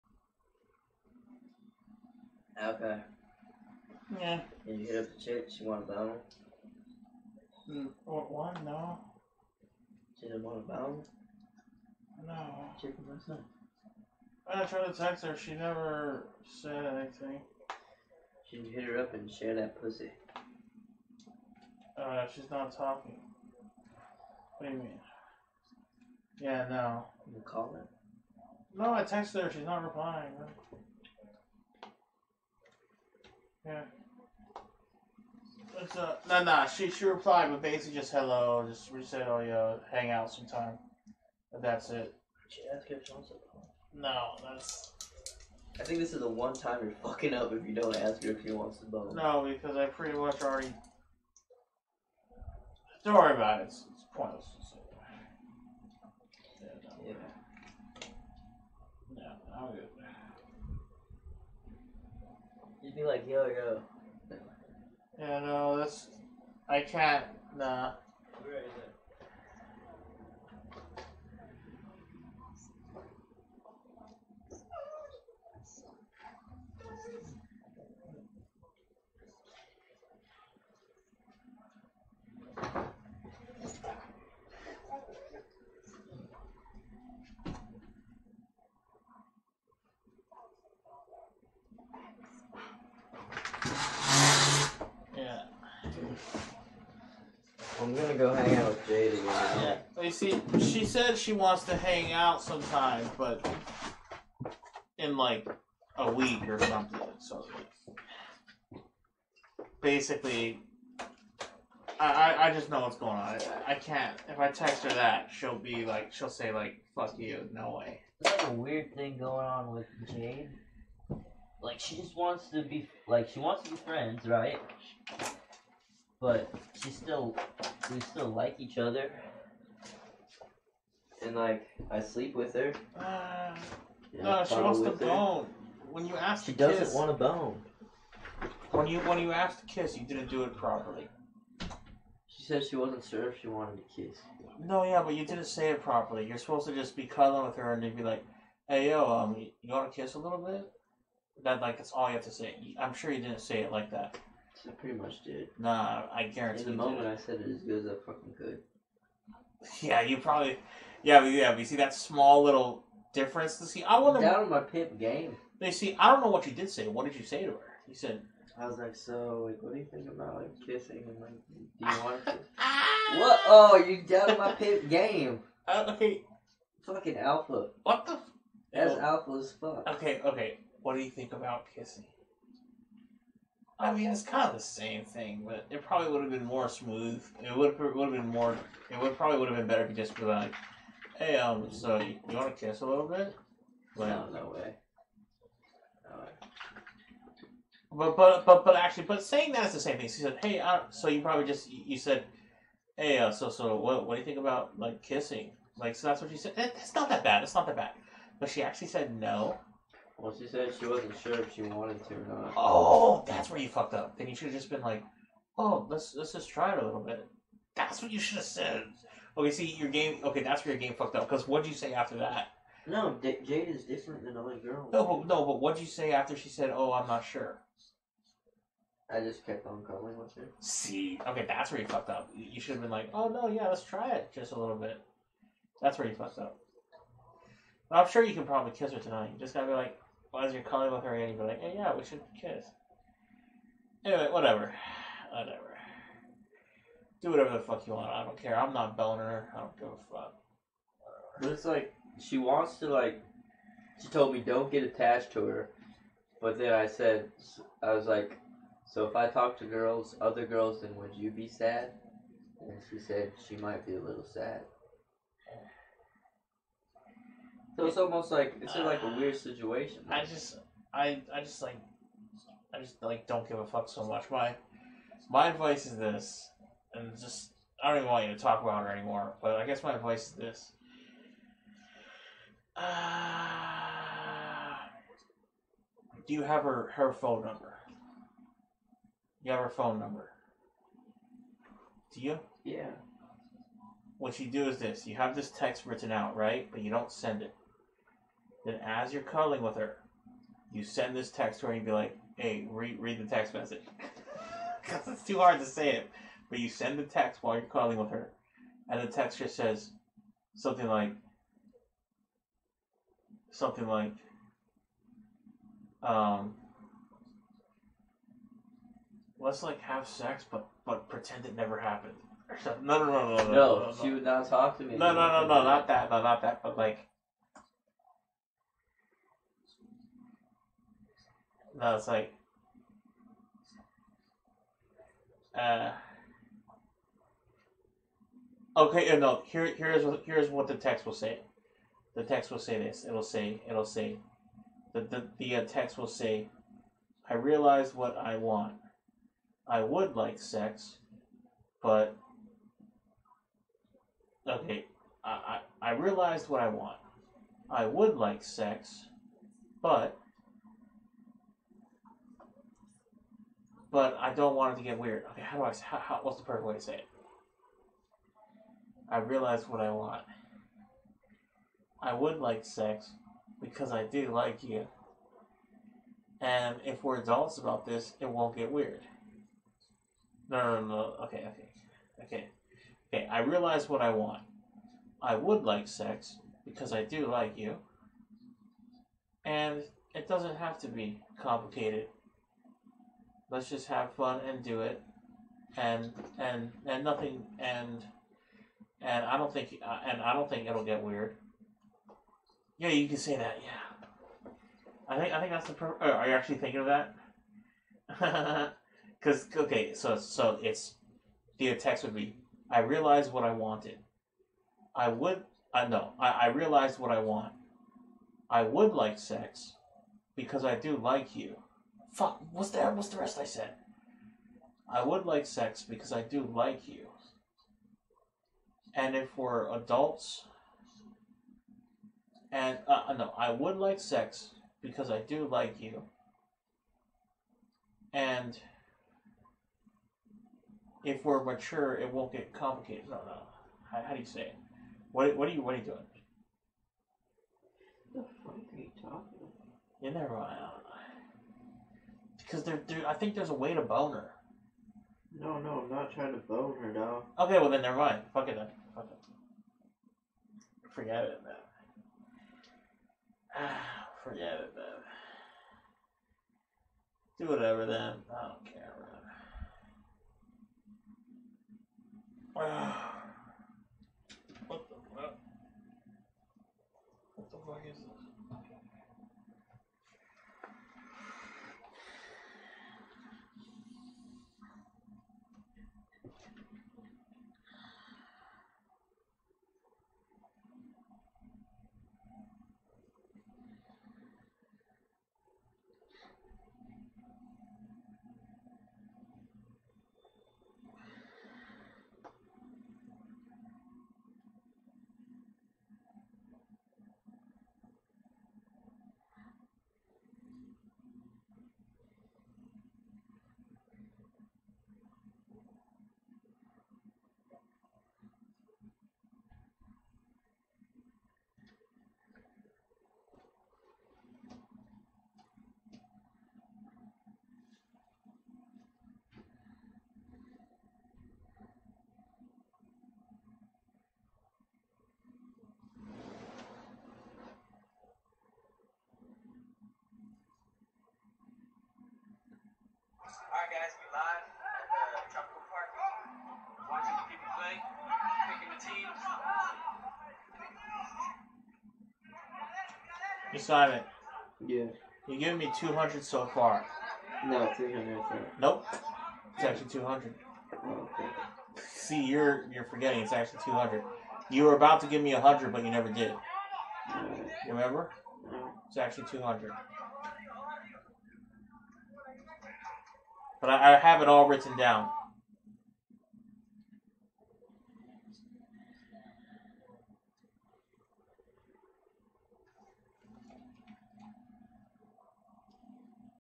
Withawonder - Advice On Girls
Withawonder - World famous Hawaiian streamer, renowned comedian & "king" offers 230 IQ advice to Simon on girls and how to get with them.